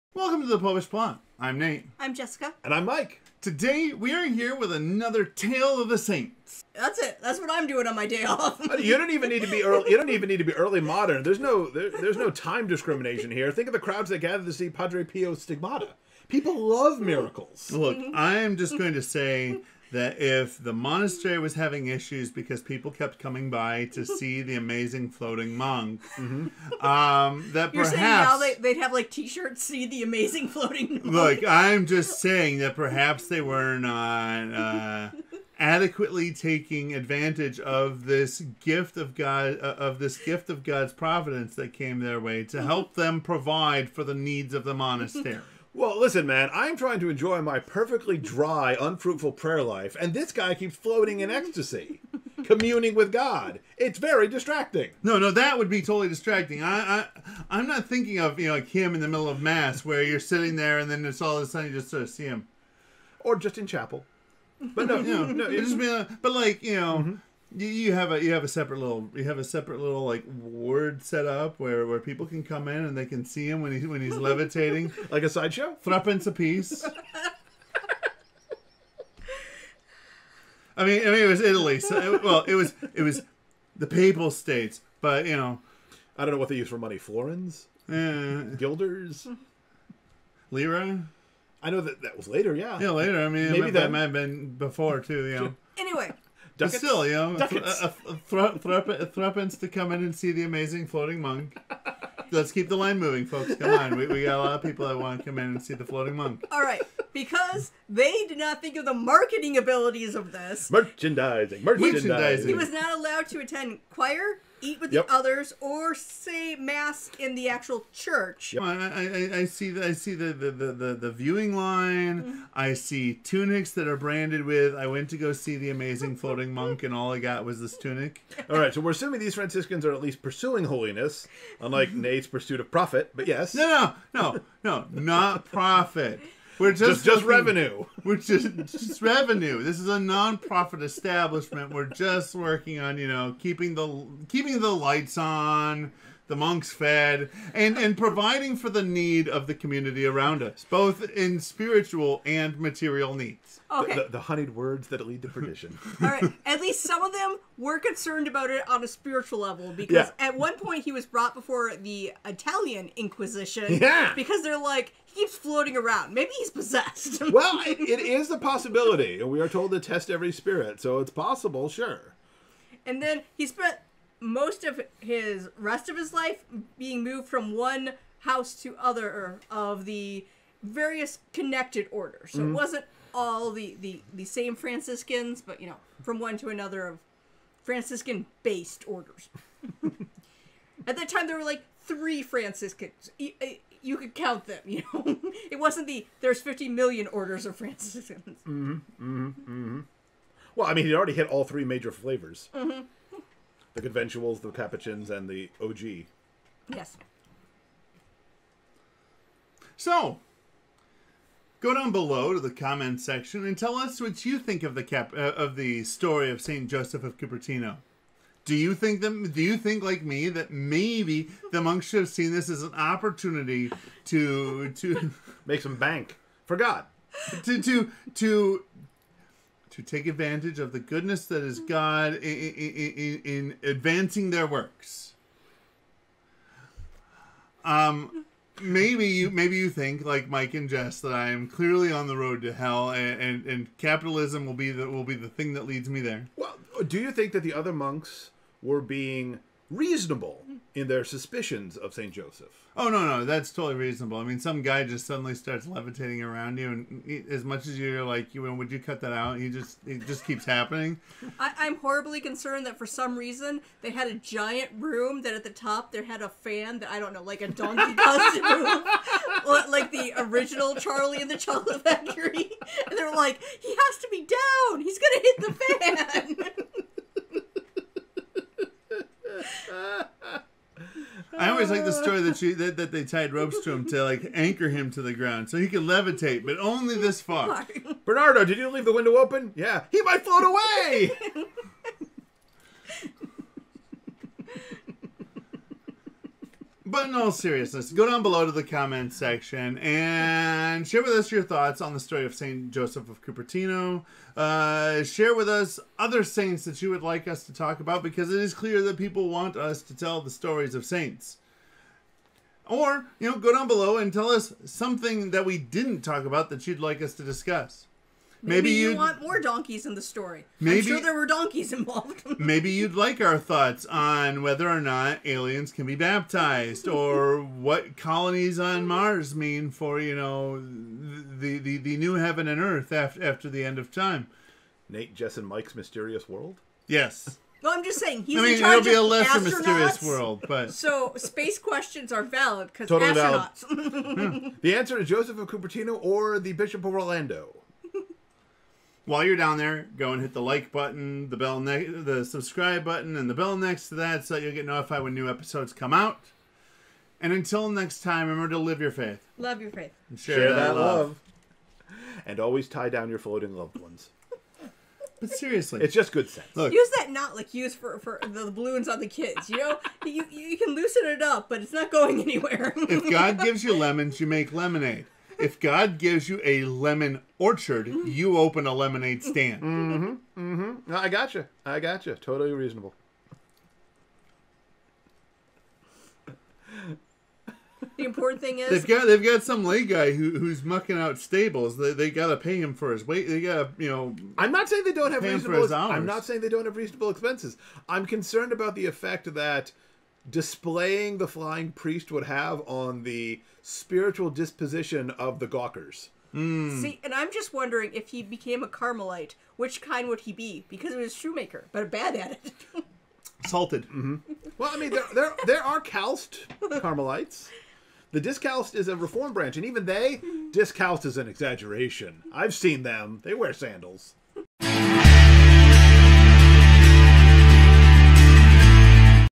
Bonus Episode: The Amazing Floating Monk
When filming "St. Joseph of Cupertino" Nate had an idea of how the monks could turn the negative of a floating monk into a positive.  It got cut for time, but here are the segments on it all put together.

Code: Plot2021